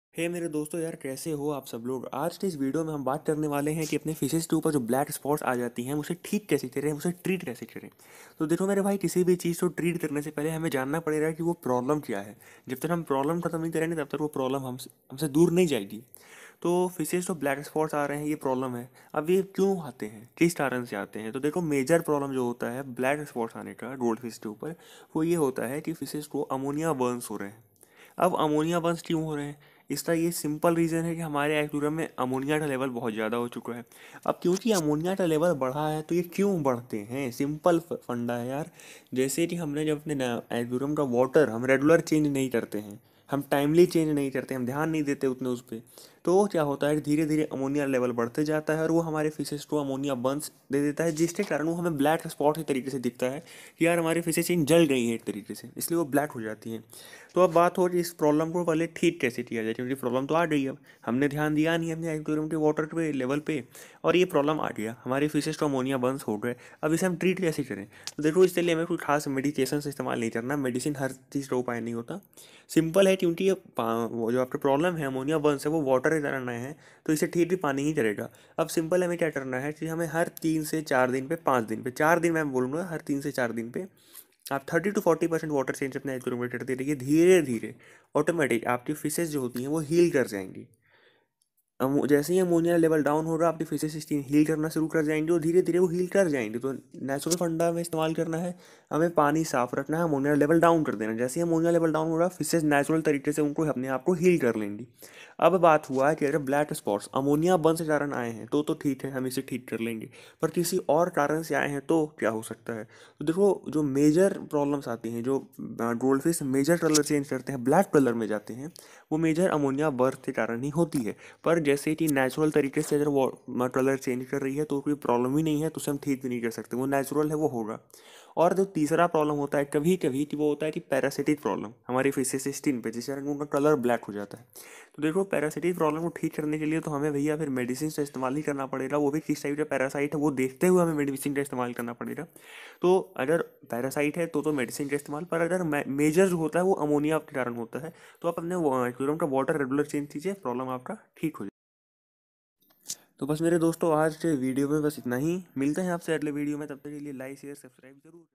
HOW TO TREAT BLACK SPOT
Hey guys in this video I have told you all the main reason for black spot and how to cure it.